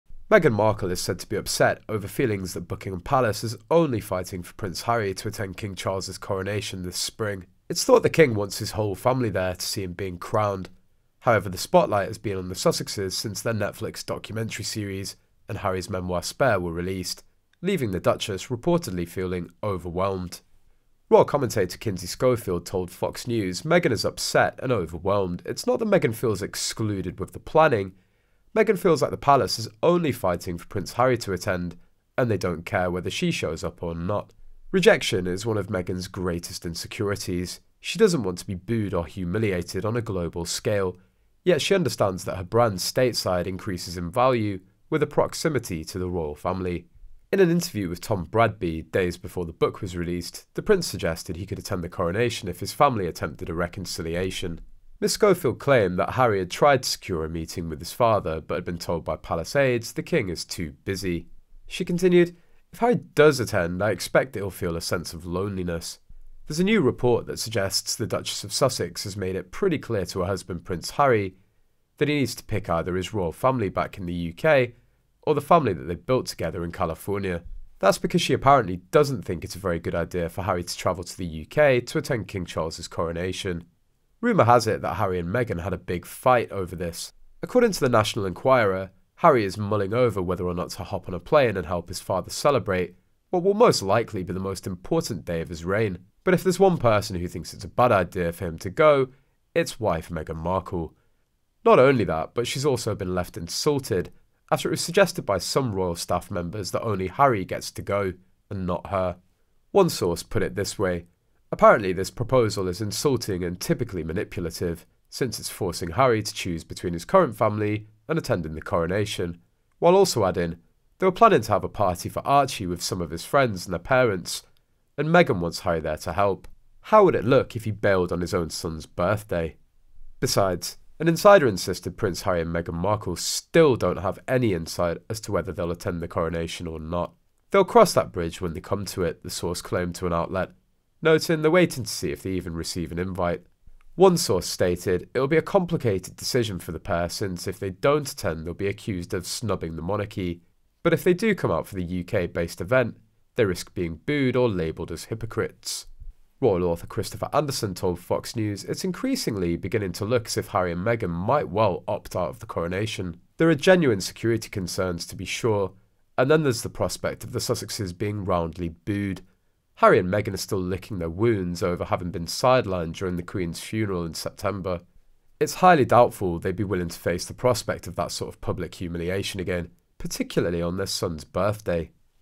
Sussex BIG FIGHT Over Coronation! Meghan SCREAMS At Harry: Choose Me Or The Royal Family
Meghan Markle is said to be "upset" over feelings that Buckingham Palace is "only fighting for Prince Harry" to attend King Charles's Coronation this spring. It is thought the King wants his whole family there to see him being crowned, however the spotlight has been on the Sussexes since their Netflix documentary series and Harry's memoir Spare were released, leaving the Duchess reportedly feeling "overwhelmed".

Royal commentator Kinsey Schofield told Fox News: "Meghan is upset and overwhelmed. It’s not that Meghan feels excluded with the planning.